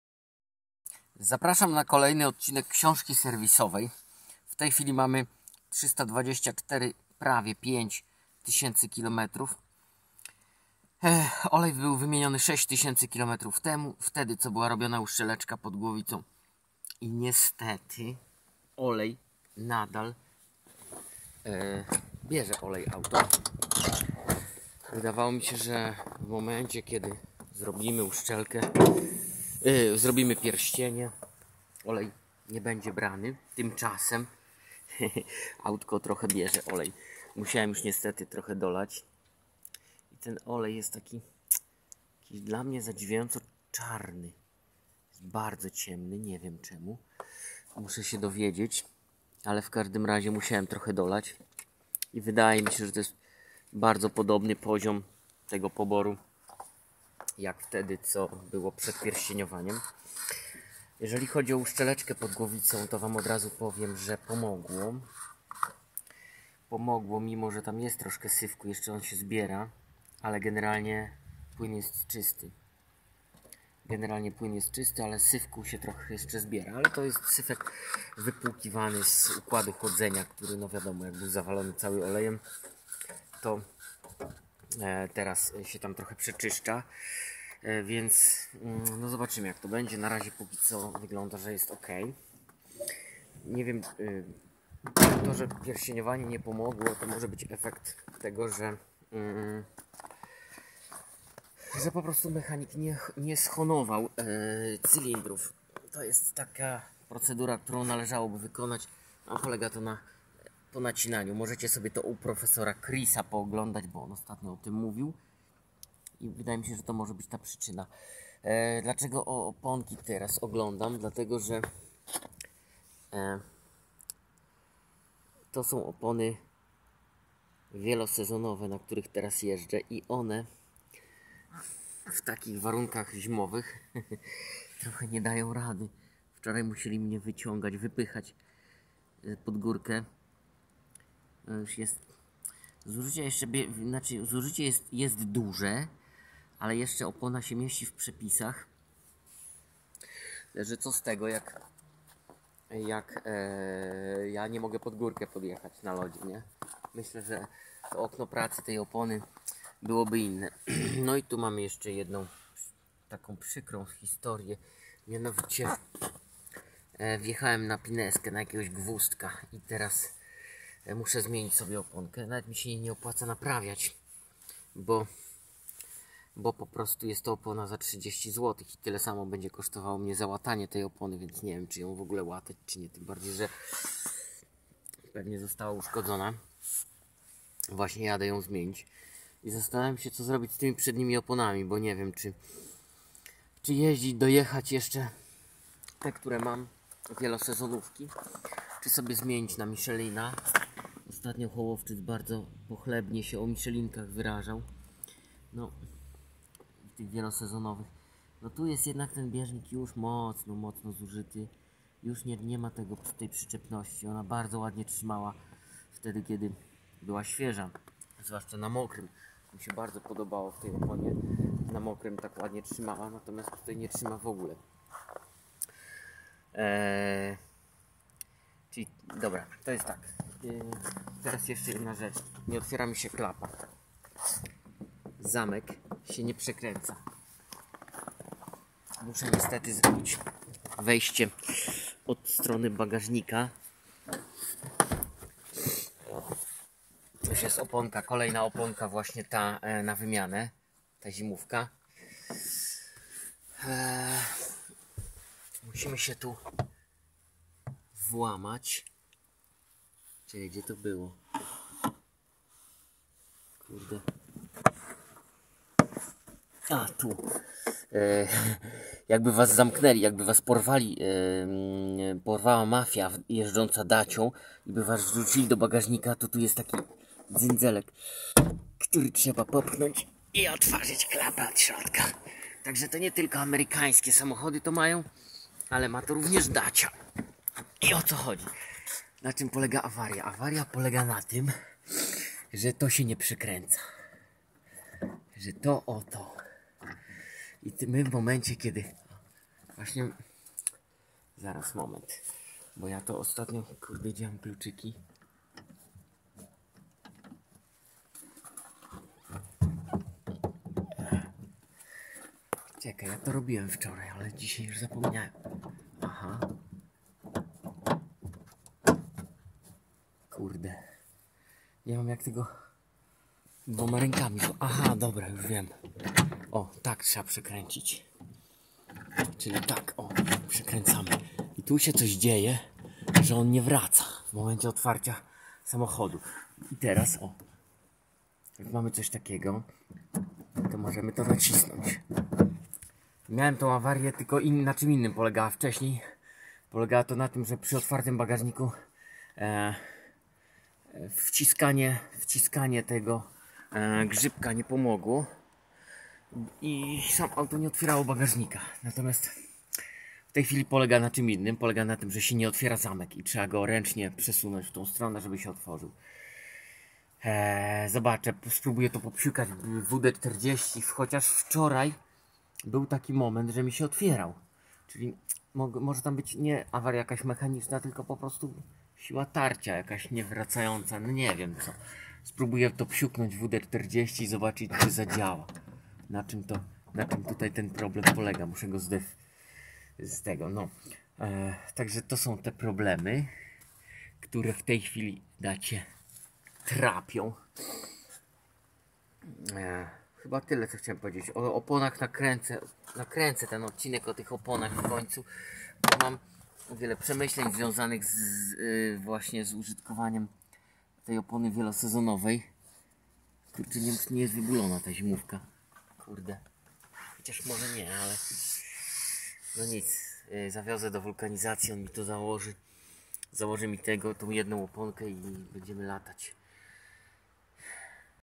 KsiążkaSerwisowaDaciaLogan# 13 / Awaria zamka tylnej klapy i dziura w oponie
Problemy z otwieraniem tylnej klapy.  Przy 320 tys. km. przy otwartym zamku, klapa nie dawala się otworzyć, a teraz nie otwiera się zamek. Powietrze schodzi z opony po najechaniu na gwóźdź.